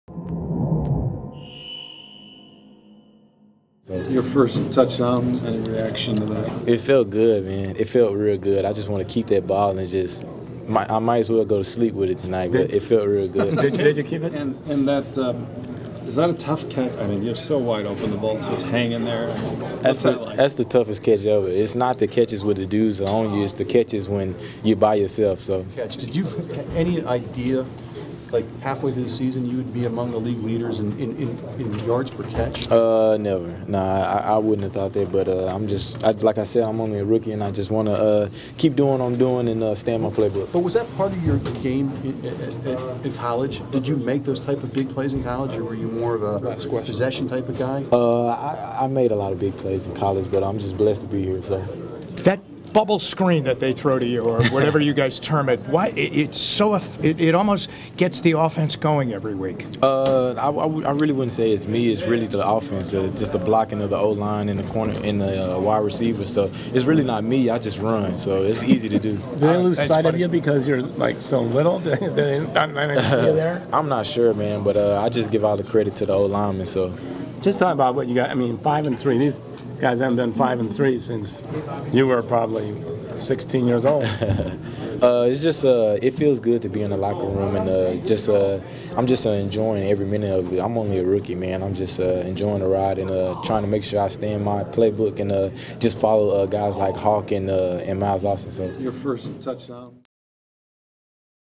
Taylor Gabriel on his first NFL touchdown in Browns win over Buccaneers
Cleveland Browns rookie wide receiver Taylor Gabriel talks in the locker room about his first NFL touchdown, which was the winning score, after their 22-17 victory over Tampa Bay Buccaneers  Sunday, Nov. 2, 2014 at FirstEnergy Stadium.